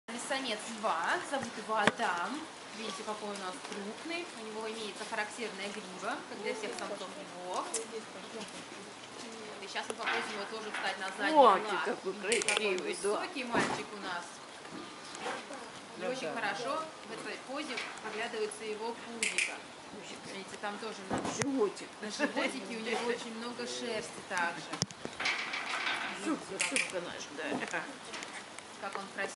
Ленинградский зоопарк. Тренинг хищников 11. Лев
Тренинг хищных кошек. Лев выполняет команды.